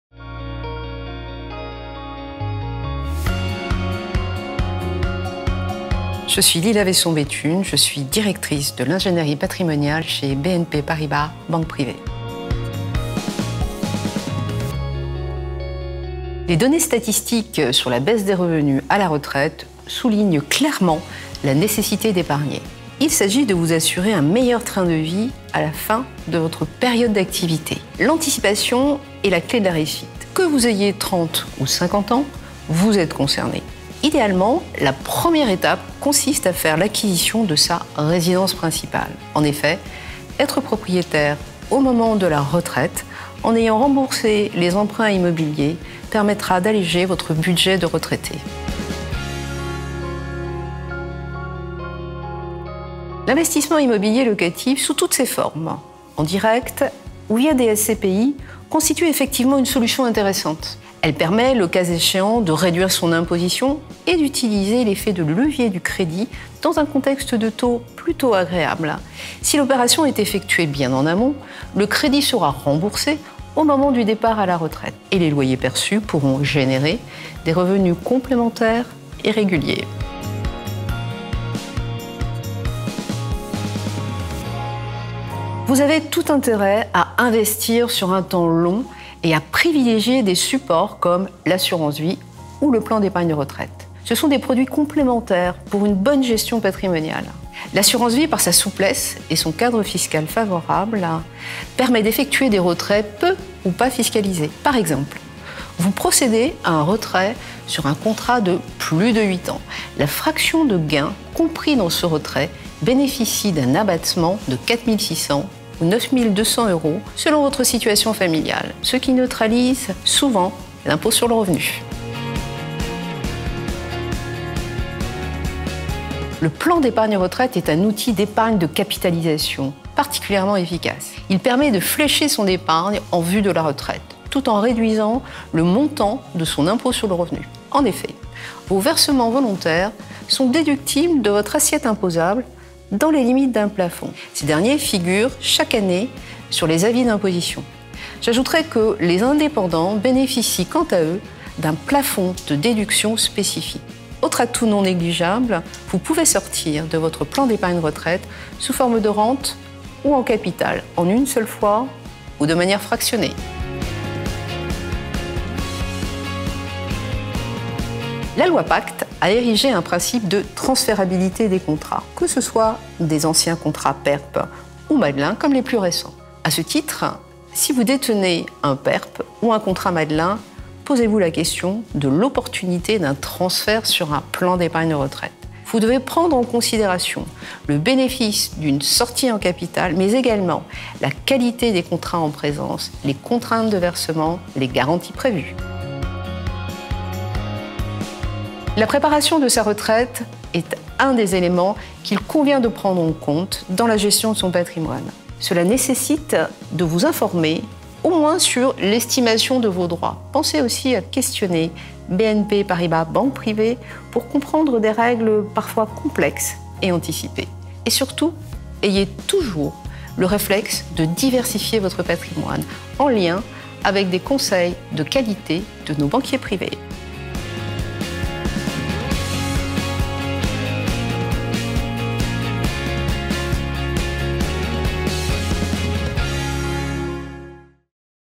LE POINT DES EXPERTS - Lila Vaisson-Bethune - Epargner pour sa retraite
Épargner pour sa retraite : le choix du PER.